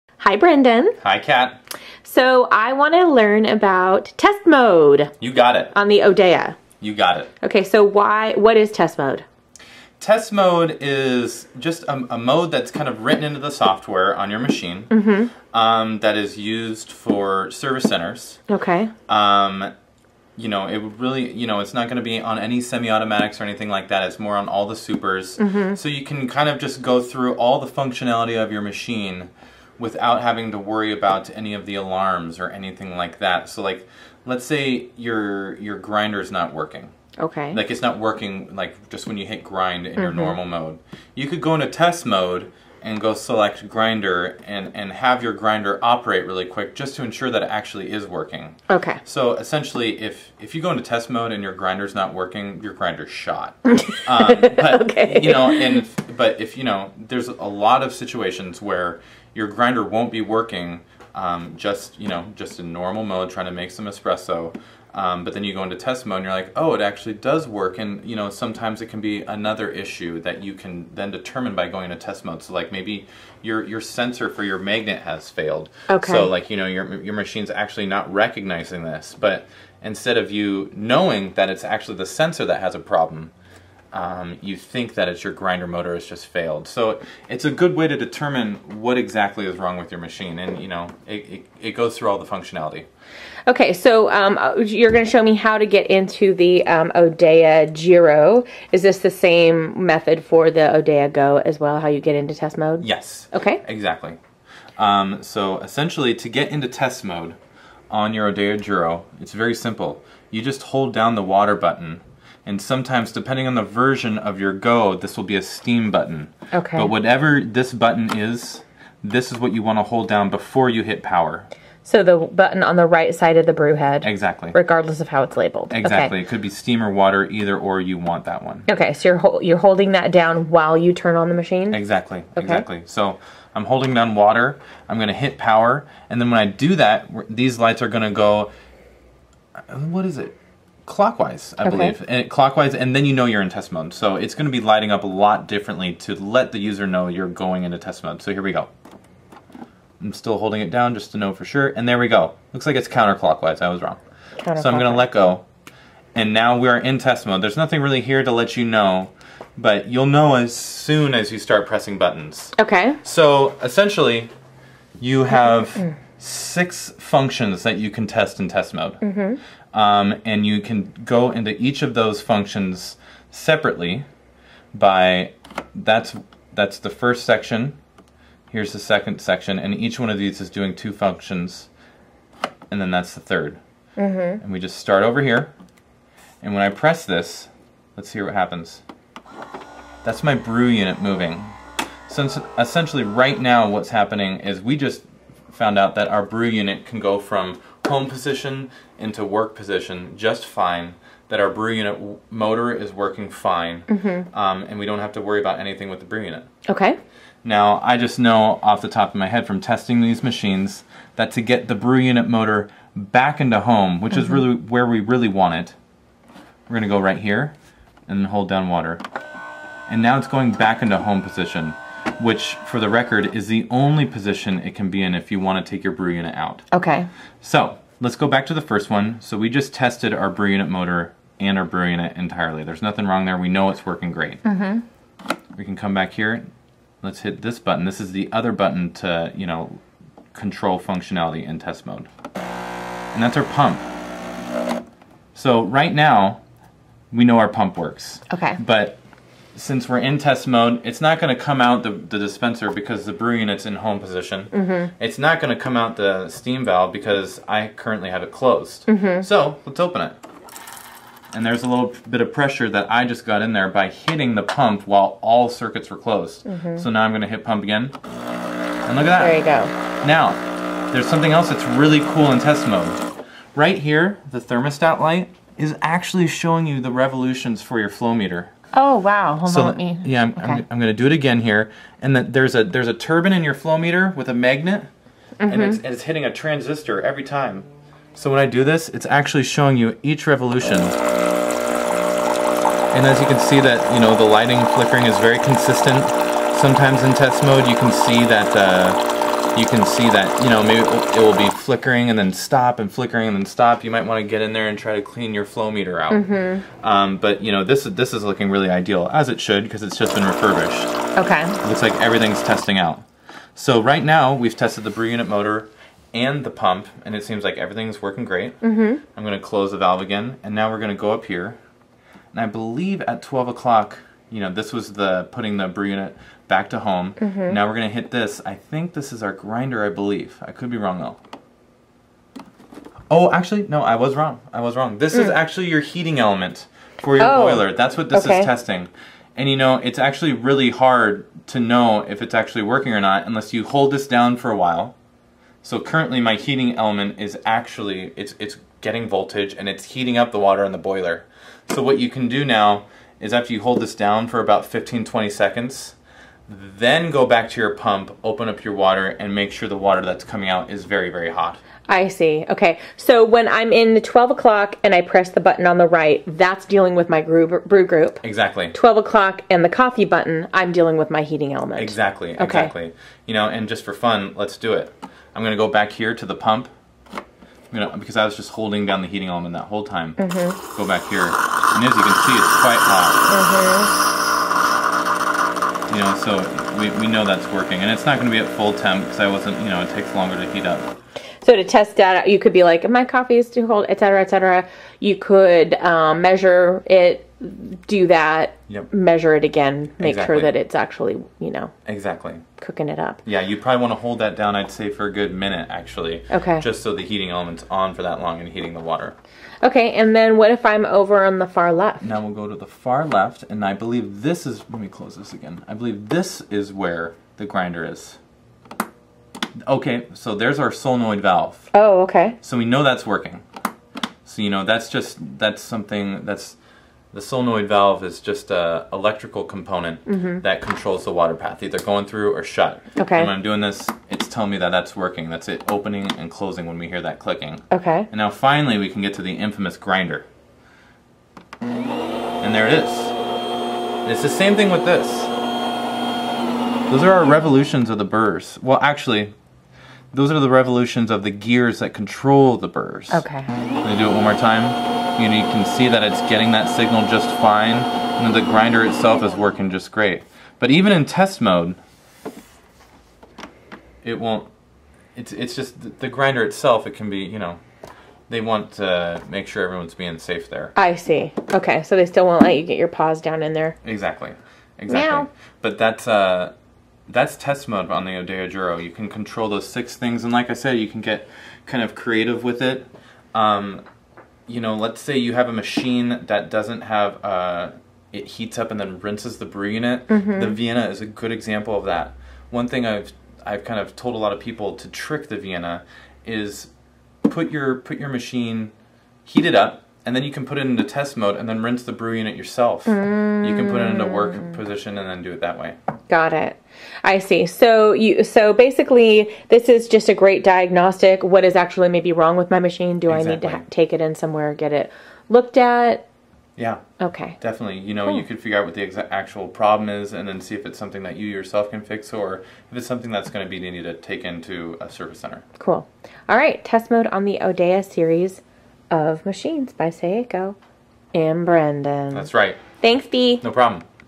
SCG Tech Tip: Saeco Odea Series Test Mode
Is your Odea series superautomatic (Go, Giro, Giro Plus, etc.) acting up? Run through a quick diagnostic of each element of the machine to troubleshoot what might be occurring. In this video, Brendan shows us how to get into test mode and then navigate through it to test each of the espresso machine's functional components.